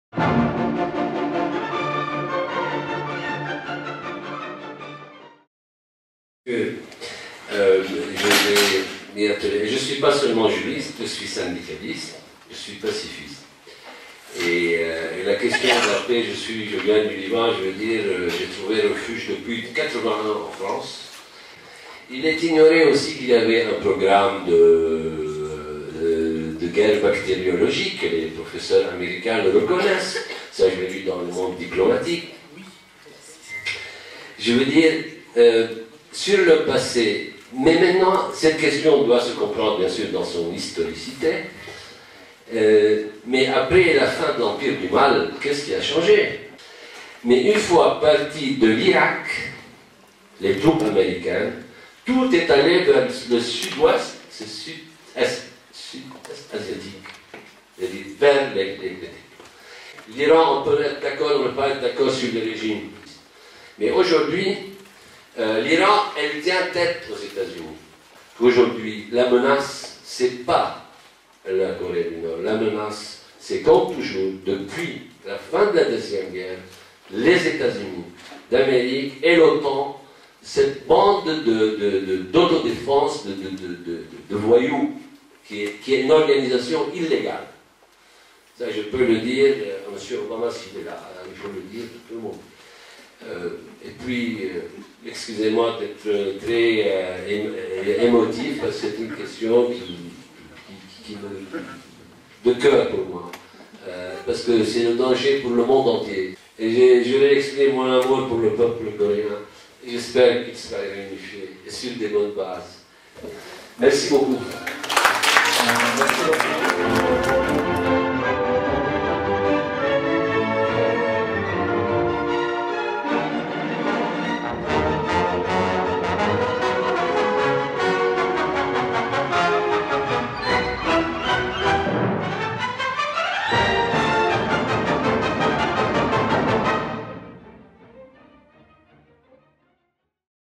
[코리아포커스] 046. 영상 : 제2회 코리아국제컨퍼런스 - 왈리드 오카이스(Walid Okais)
제2회 코리아국제컨퍼런스
2ème Colloque International sur la Corée

주제 : 코리아의 평화
Sujet : Paix en Corée

1차토론 - 코리아의 정세와 평화
1ère table-ronde - Situation et paix dans la péninsule coréenne

발표자 : 왈리드 오카이스
Intervenants : Walid Okais
노동법률가, 프랑스노동총동맹(CGT)
Juriste, CGT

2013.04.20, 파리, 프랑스
Maison des Mines, Paris